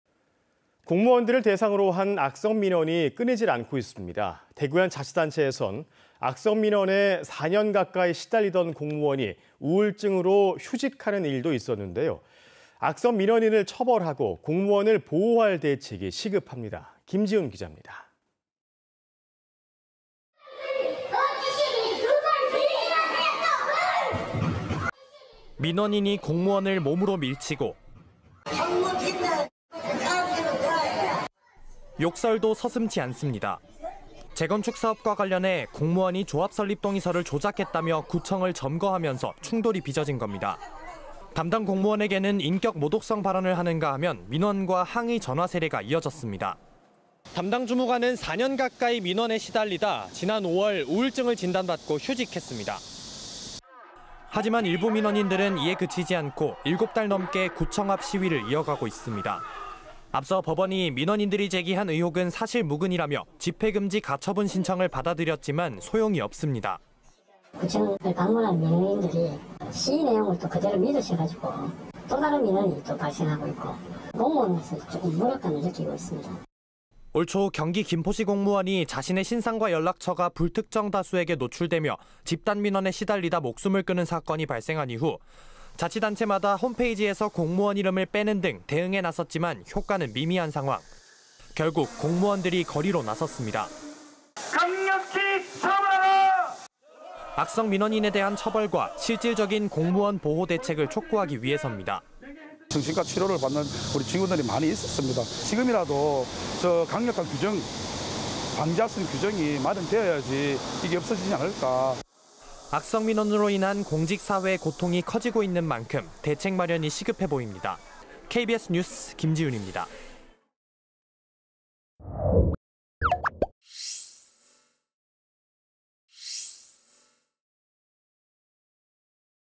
끊이지 않는 악성 민원…“처벌 규정 마련해야” / KBS 2024.08.07.
[앵커]

 공무원들을 대상으로 한 악성 민원이 끊이질 않고 있습니다.

 대구의 한 자치단체에서는 악성 민원에  4년 가까이 시달리던 공무원이 우울증으로 휴직하는 일도 있었는데요.

 악성 민원인을 처벌하고 공무원을 보호할 대책이 시급합니다.

 김지훈 기자입니다.

 [리포트]

 ["밀지 마세요 좀!"]

 민원인이 공무원을 몸으로 밀치고,

 ["총무팀장은 XX 고함 질러도 되나?"]

 욕설도 서슴지 않습니다.

 재건축사업과 관련해 공무원이 조합설립동의서를 조작했다며 구청을 점거하면서  충돌이 빚어진 겁니다.

 담당 공무원에게는 인격 모독성 발언을 하는가 하면, 민원과 항의 전화 세례가 이어졌습니다.

 담당 주무관은 4년 가까이 민원에 시달리다 지난 5월 우울증을 진단받고 휴직했습니다.

 하지만 일부 민원인들은 이에 그치지 않고 7달 넘게 구청 앞 시위를 이어가고 있습니다.

 앞서 법원이 민원인들이 제기한 의혹은 사실 무근이라며 집회금지 가처분 신청을 받아들였지만 소용이 없습니다.

 [달서구청 관계자/음성변조 : "구청을 방문하는 민원인들이 시위 내용을 또 그대로 믿으셔서 또 다른 민원이 발생하고 있고…. 공무원으로서 조금 무력감을 느끼고 있습니다."]

 올초 경기 김포시 공무원이 자신의 신상과 연락처가 불특정 다수에게 노출되며 집단 민원에 시달리다 목숨을 끊은 사건이 발생한 이후, 자치단체마다 홈페이지에서 공무원 이름을 빼는 등 대응에 나섰지만 효과는 미미한 상황, 결국, 공무원들이 거리로 나섰습니다.

 ["강력히 처벌하라! (처벌하라! 처벌하라! 처벌하라!)"]

 악성 민원인에 대한 처벌과  실질적인 공무원 보호 대책을  촉구하기 위해섭니다.

 [김규환/공무원노조 달서구지부장 : "정신과 치료를 받는 우리 직원들이 많이 있었습니다. 지금이라도 강력한 규정이, 방지할 수 있는 규정이 마련돼야지 이게 (악성 민원이)없어지지 않을까…."]

 악성 민원으로 인한 공직사회의 고통이 커지고 있는 만큼, 대책 마련이 시급해 보입니다.

 KBS 뉴스 김지훈입니다.

 촬영기자:신상응/화면제공:대구 달서구청